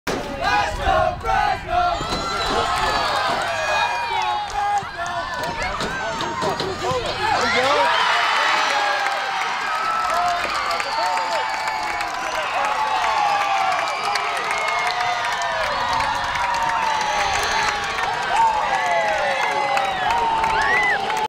Fresno State Women's Soccer: Flip Throw-in Assist for a Goal
Fresno State freshman Chloe Chedester delivers the front flip throw-in assist to junior Tori Nicolo for the game-tying goal in the Bulldogs' 2-2 draw against Sacramento State.